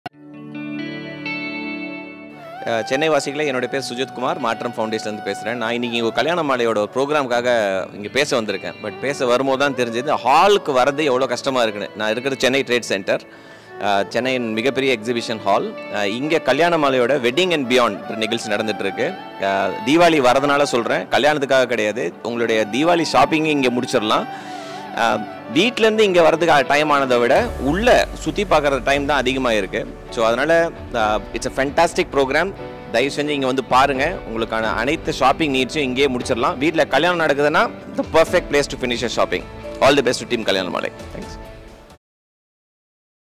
Mr. Sujith Kumar from Maatram Foundation
The event is absolutely electrifying!

But don't just take our word for it. Listen to Mr. Sujith Kumar from Maatram Foundation as he shares his firsthand experience at the grandest carnival in the city.

Hurry, it's the last day today! Join us and be part of the this amazing experience!